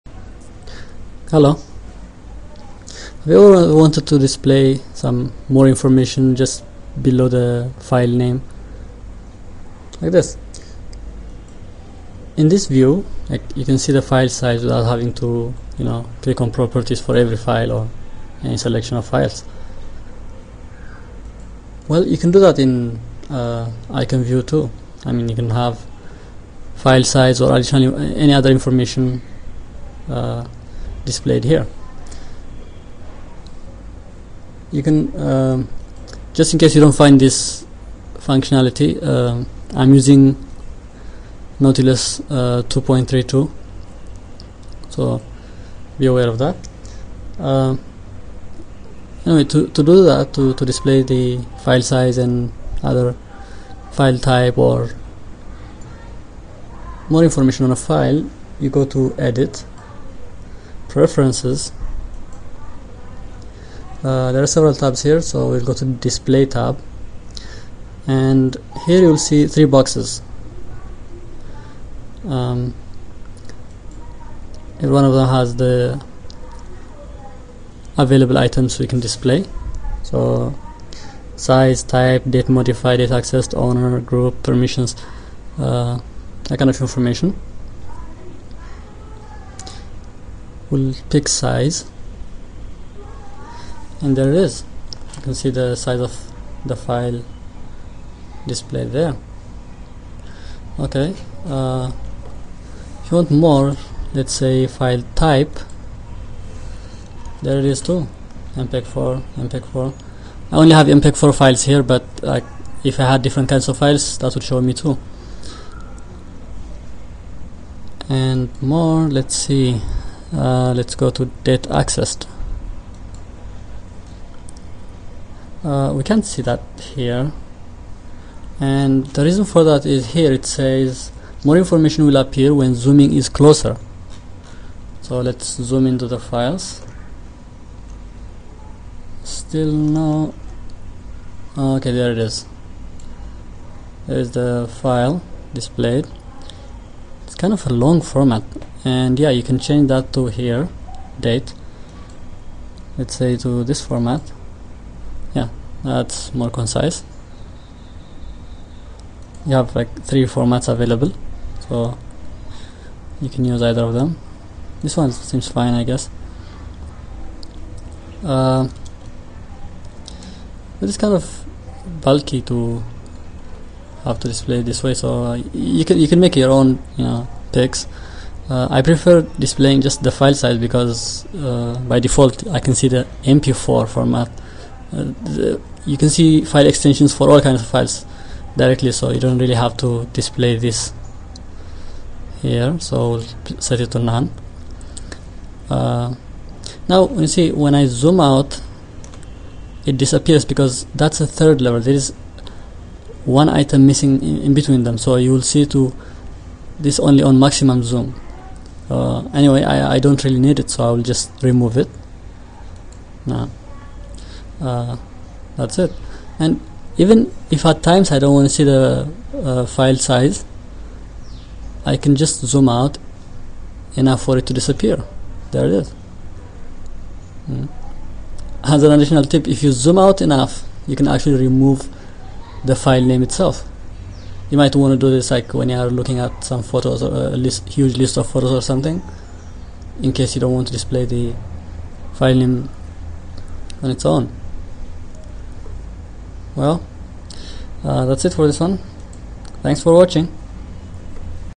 Displaying More Info below on Icon View - QuickTip
How to display more info on icon view below the filename in Nautilus.